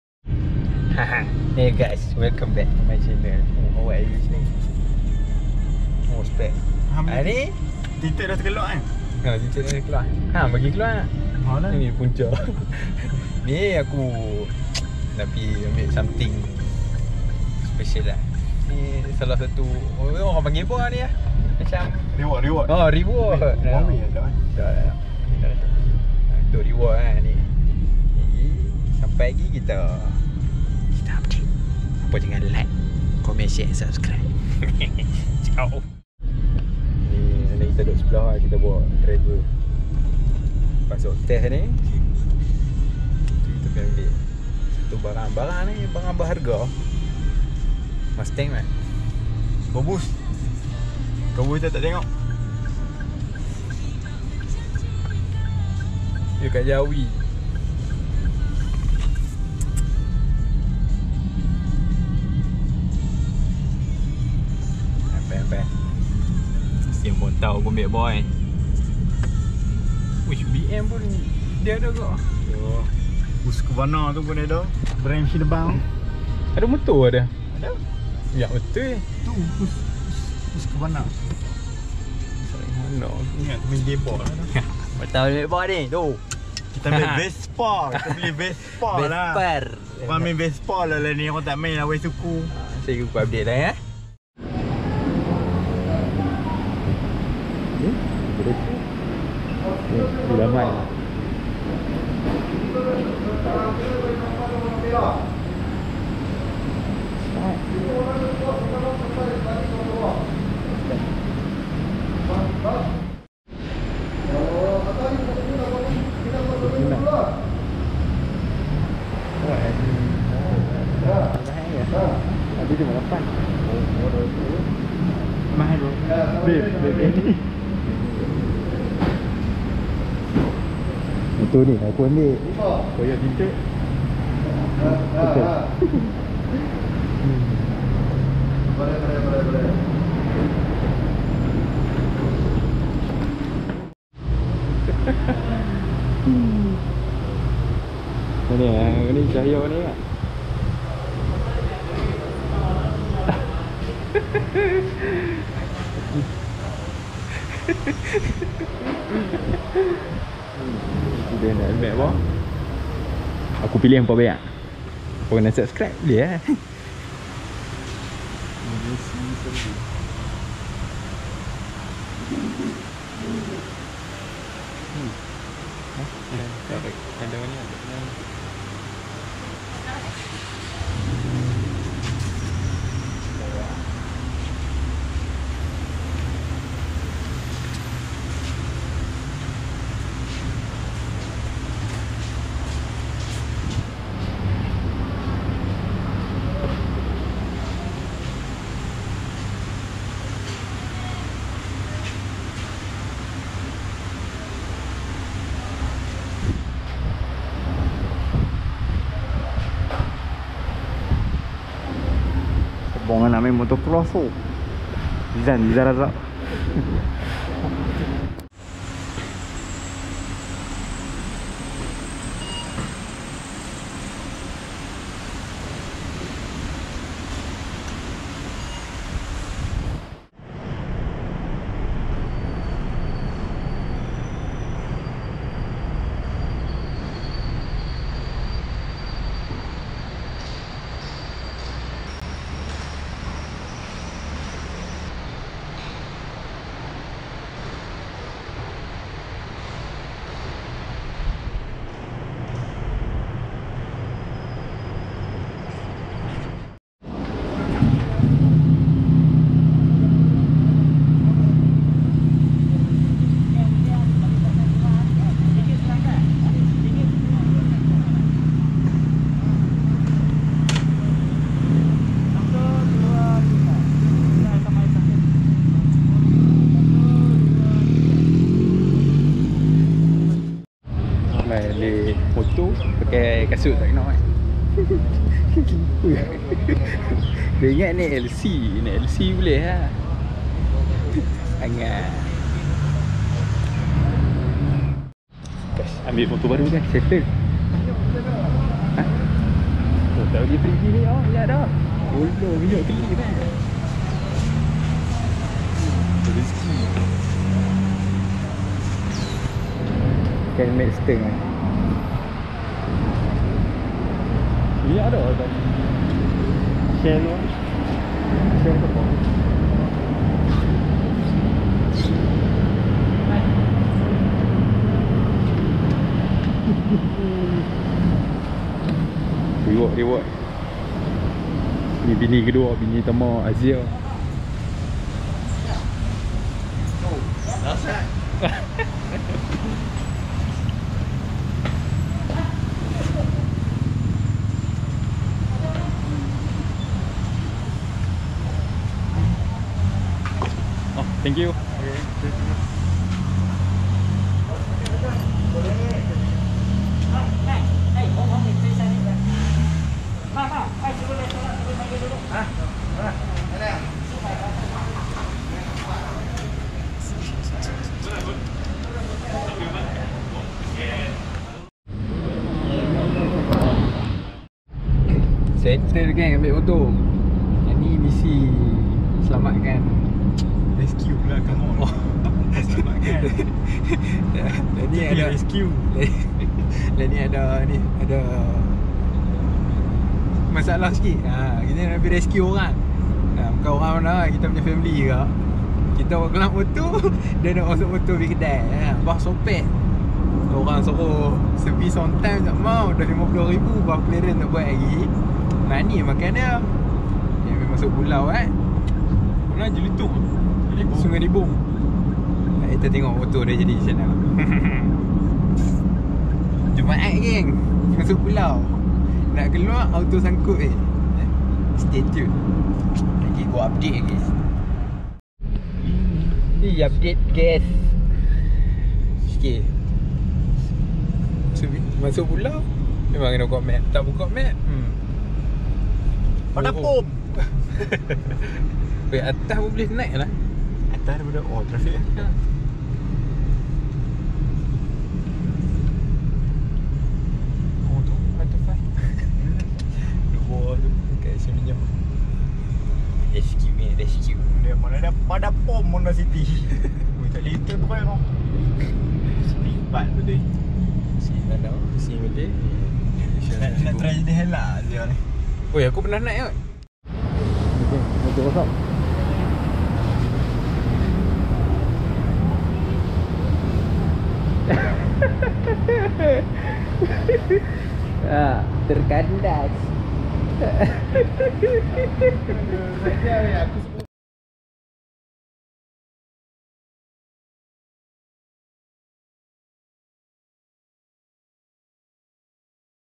AKU BELI MOTO BARU | yamaha r25 | vlog
Tq geng yang mna support aku..moga hampa murah rezeki dunia akhirat. Ameen

Kedai moto : el chong jawi